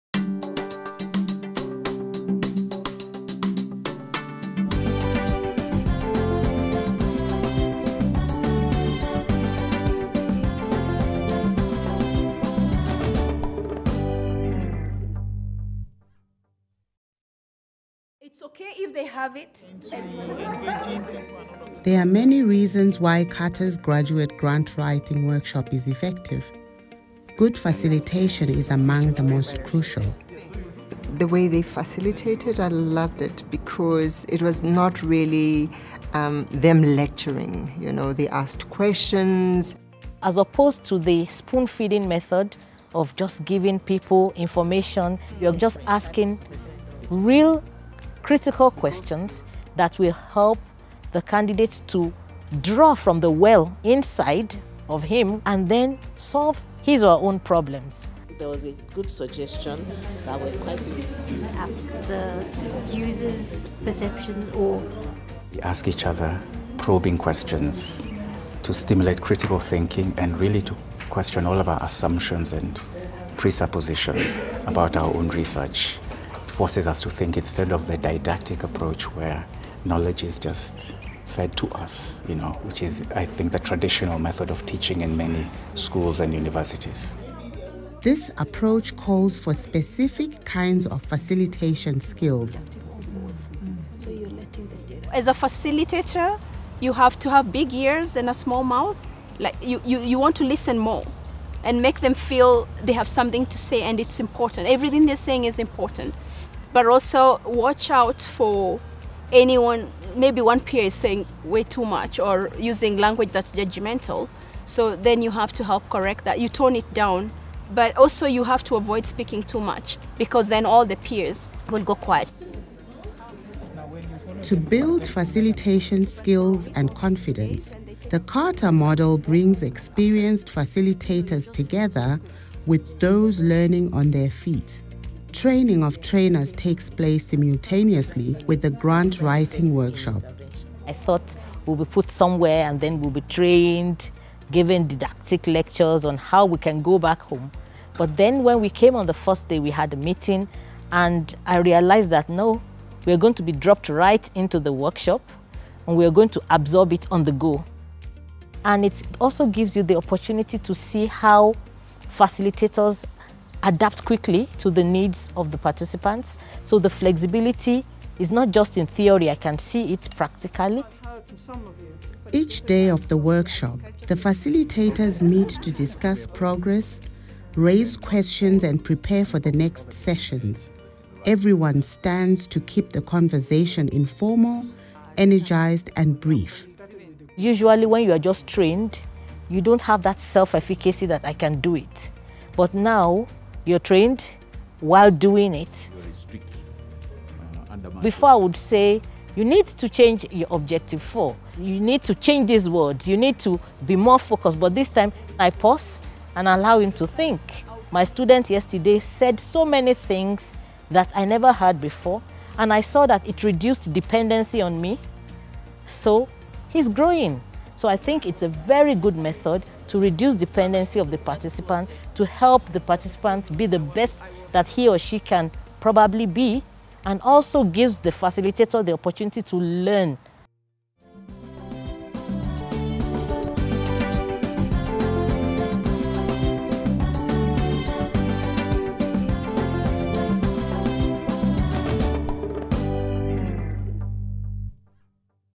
"Big ears and a small mouth": Facilitating a Grant Writing Workshop
To facilitate CARTA’s model of a grant writing workshop requires a responsive, participatory approach, engaging participants in active learning-by-doing. Newer facilitators learn in the process of the workshop itself, with guidance from experienced colleagues. - spells out in detail how to facilitate and how to train new facilitators.